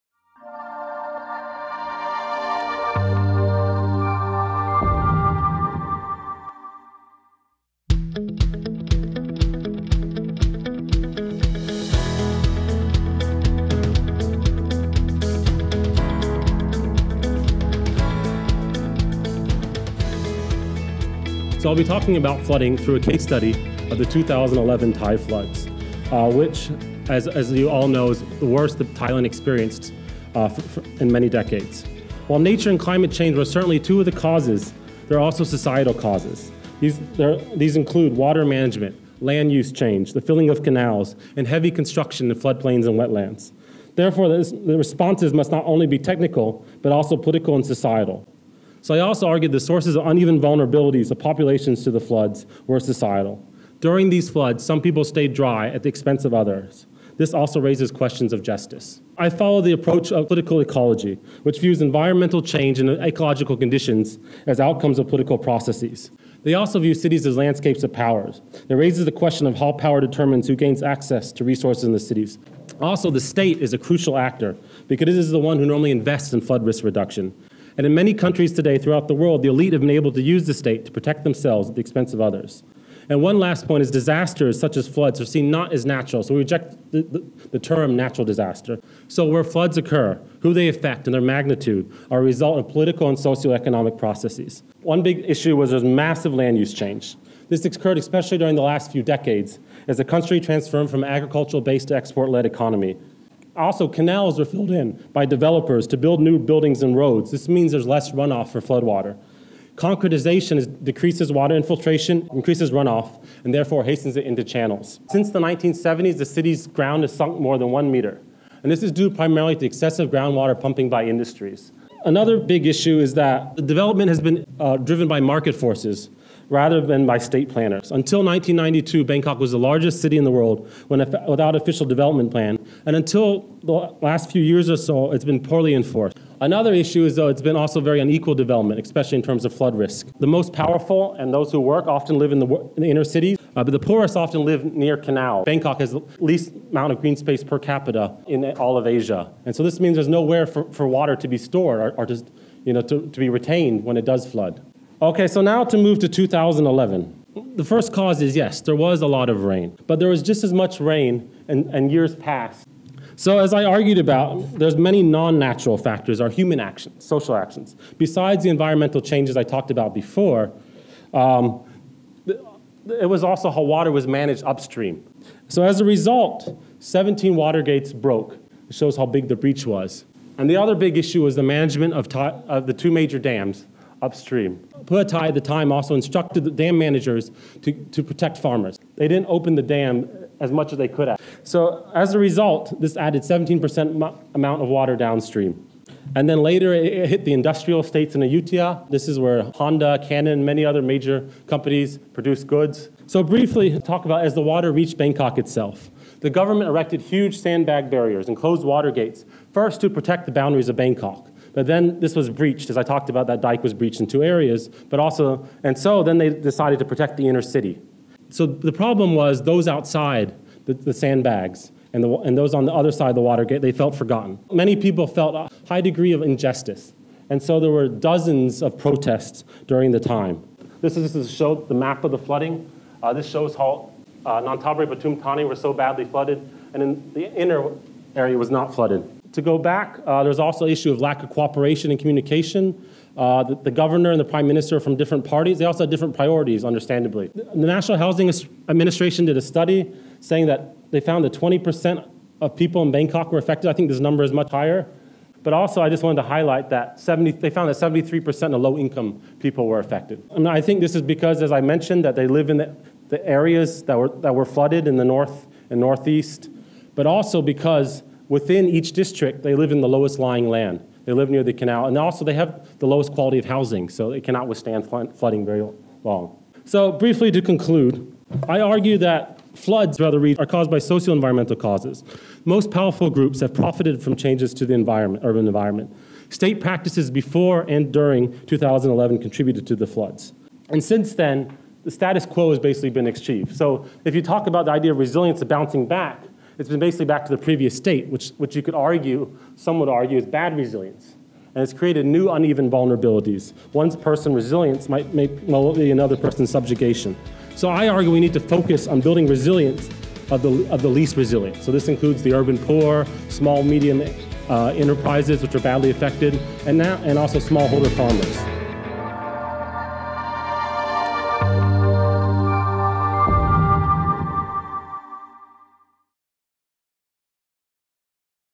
Climate Resilience and Capacity to Respond: Case Study of Thailand
This purpose of the series of talks held on October 6, 2016 was to facilitate an exchange with the larger development community to take stock of the collective knowledge, data, tools, information, frameworks, and lessons learned in assessing and addressing climate risks and incorporating climate-resilience considerations into development activities at large.

The sub-theme discussed by Danny Marks in the above video is: Climate Resilience and Capacity to Respond: Case Study of Thailand.